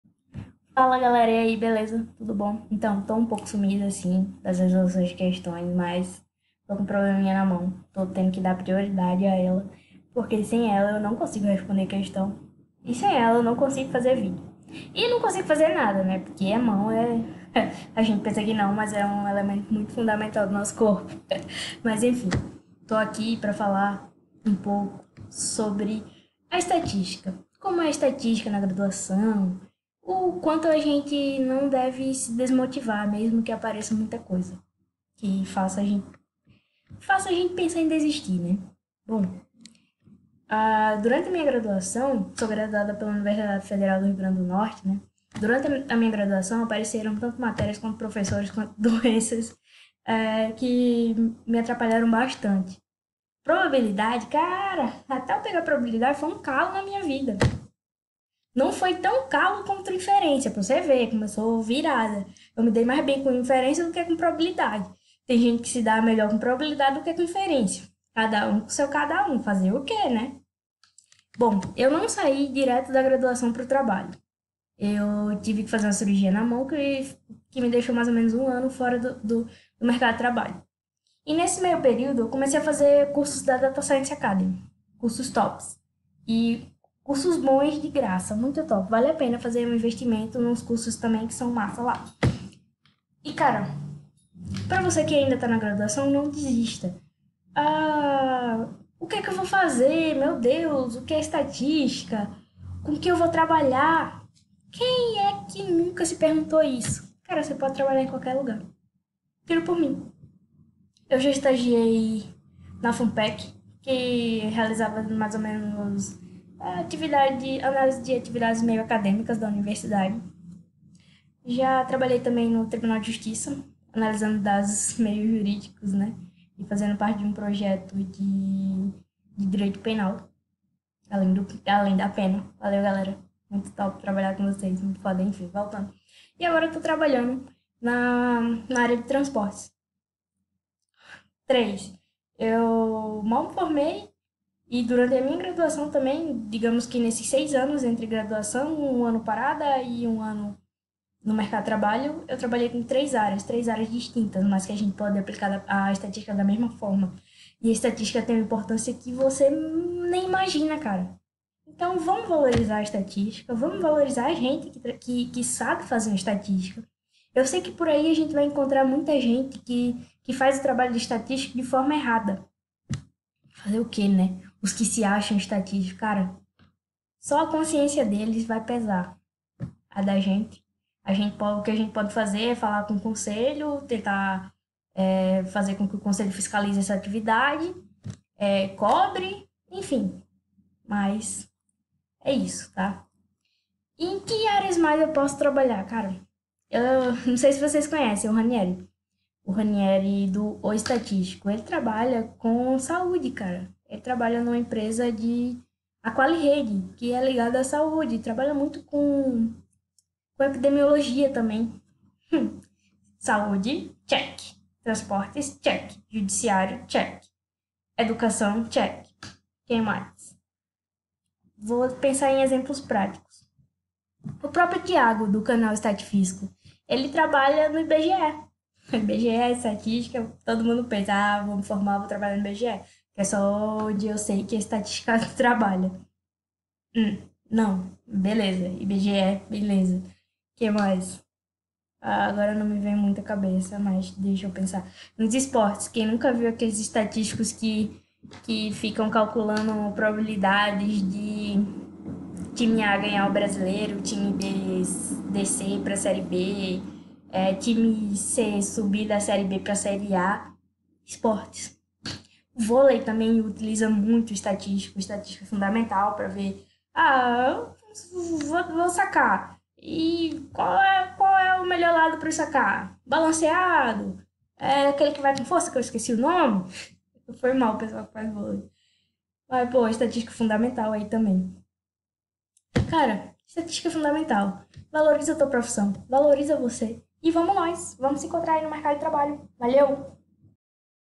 Vídeo motivacional - Professora Gabi
Neste vídeo a professora Gabi faz um vídeo motivacional aos estudantes de estatística e também quem deseja trabalhar na área.